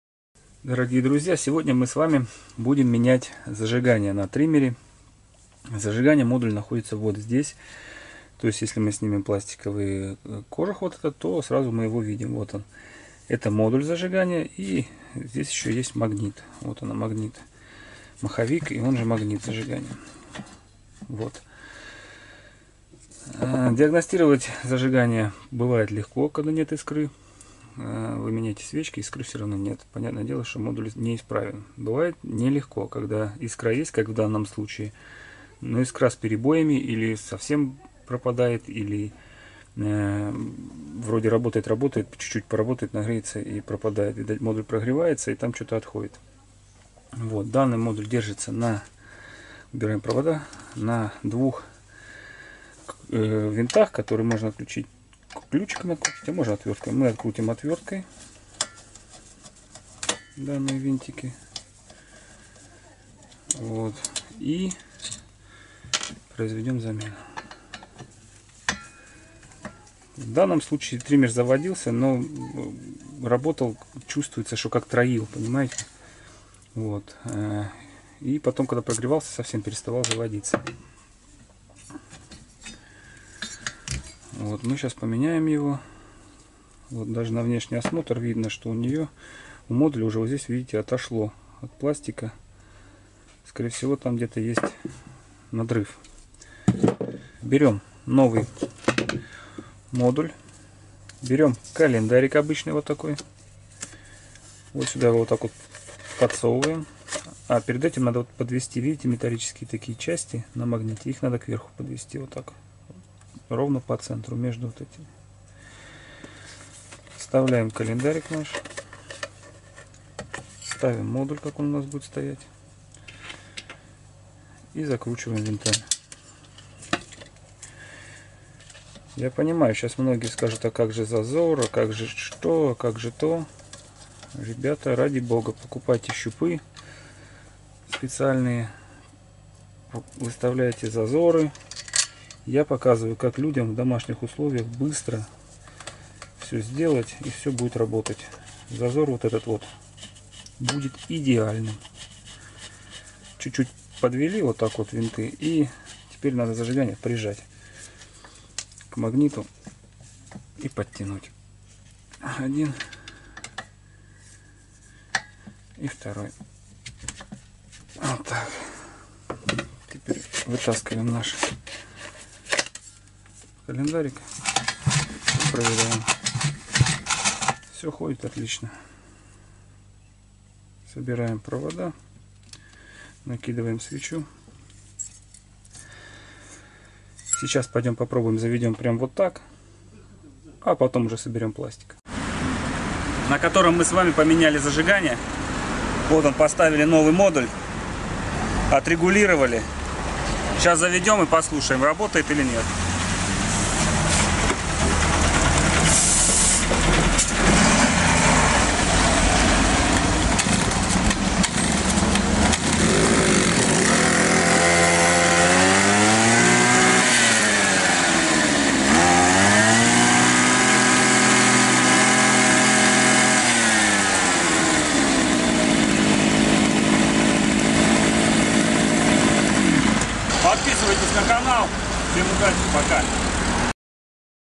Как заменить зажигание на триммере?
Видео поможет поменять зажигание на триммере, зажигание на триммере можно поменять самостоятельно, для этого не нужно каких то специальных приборов и приспособлений.Я покажу как просто и быстро поменять зажигание на триммере самостоятельно.Еще больше информации о ремонте и выборе инструмента на нашем сайте заходите мы рады каждому.